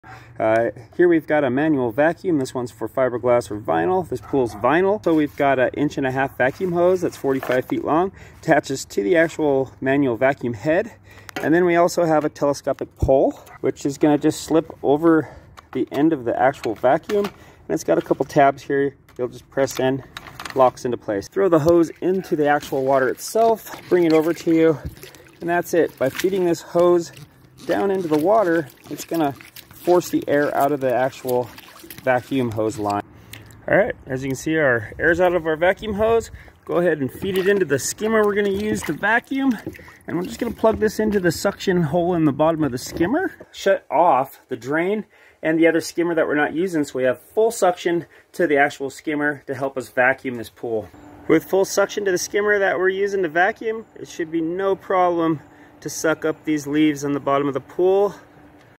How to Vacuum a Pool with manual vacuum
A Quick on Preparing a Manual Vacuum For a Swimming Pool
Vinyl Pool Vacuum
Pool Vacuum Hose